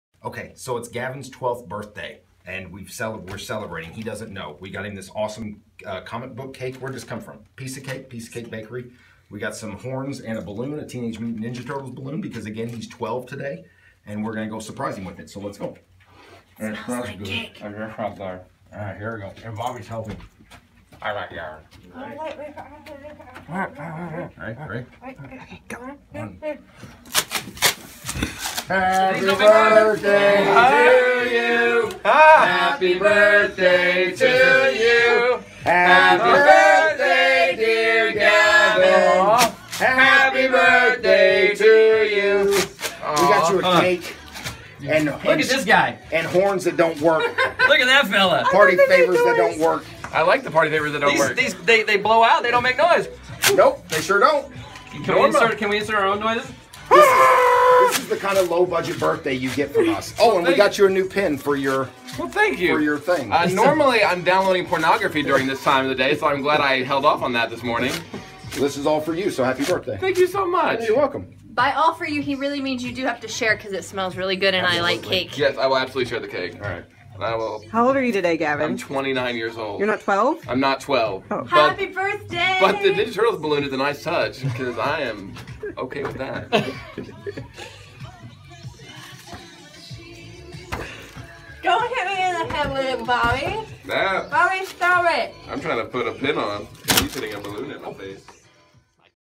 Surprising Gavin with Cake on His Birthday
Gavin turned 29, so we decided to surprise him with a cake that a 12 year old (and Gavin) would love.